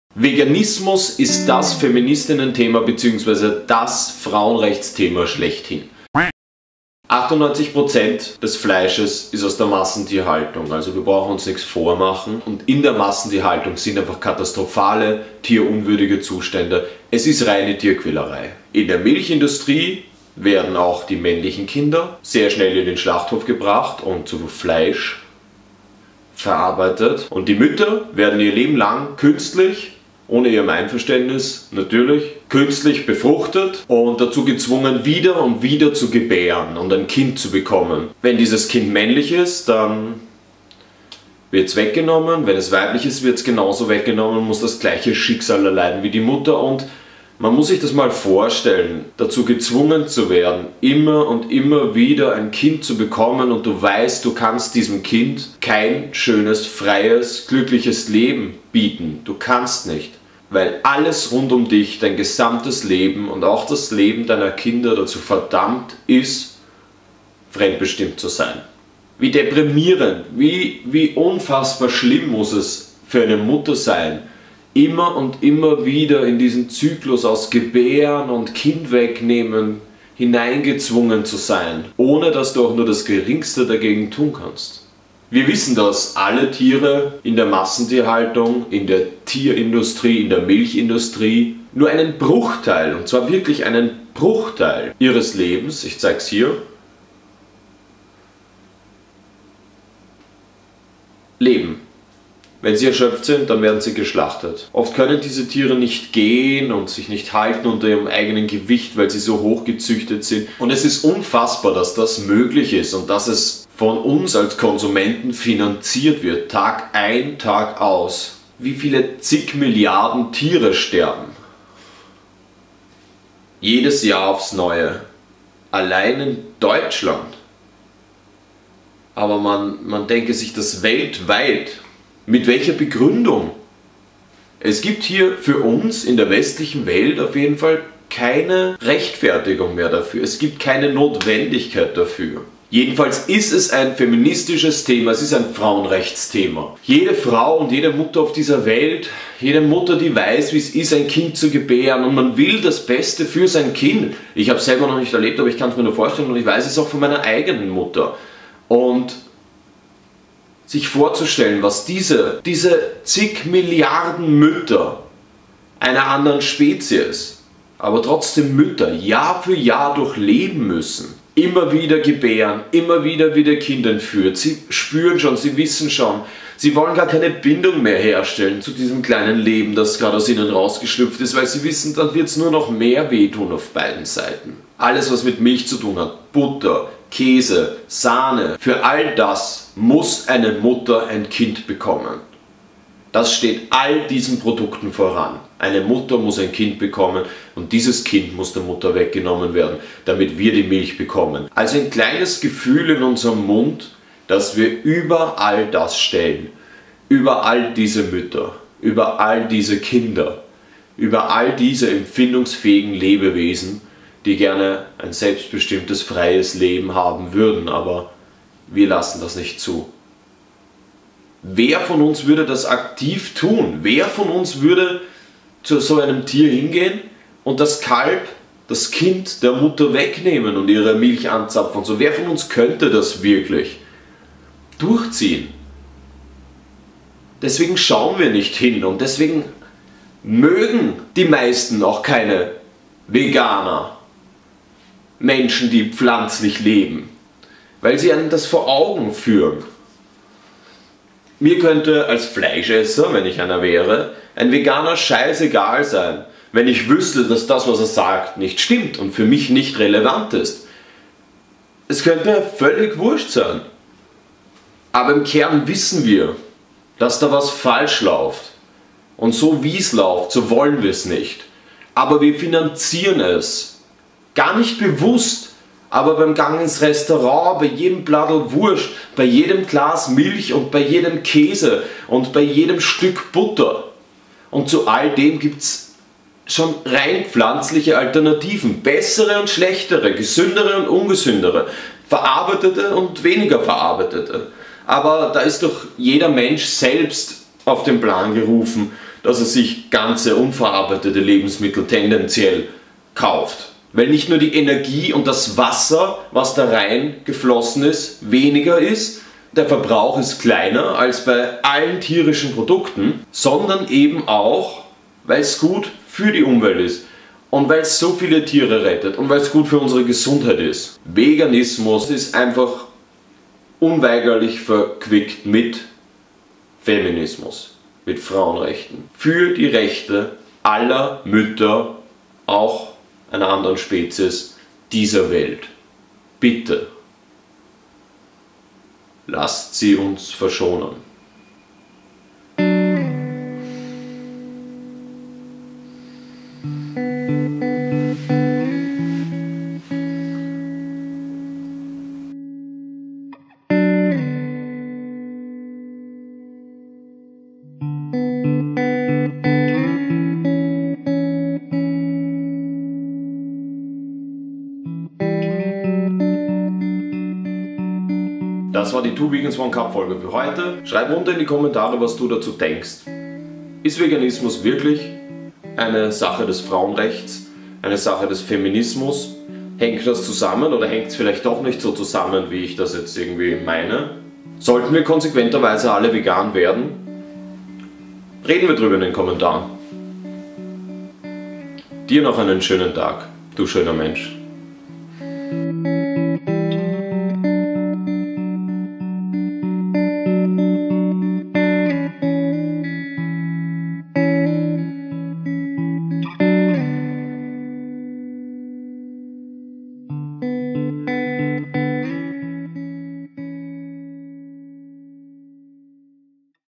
✊ Frauenrechte/Feminismus: Unweigerlich mit Veganismus verlinkt?
Feminismus ist vegan. Oder täusche ich mich?